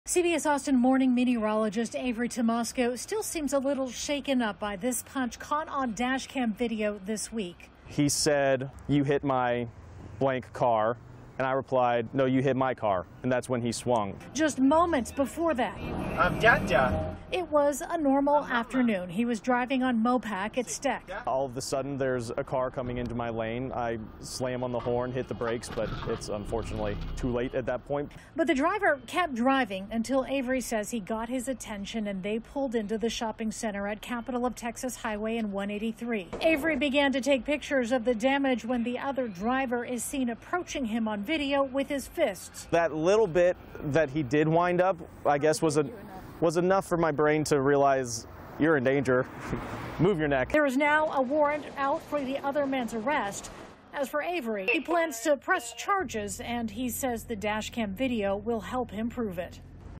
Road rage attack captured on dash cam of CBS Austin meteorologist
Earlier this week, CBS Austin's morning meteorologist shared a video of a road rage altercation. Avery Tomasco posted the video to show how volatile these interactions can be and to vouch for dashcams.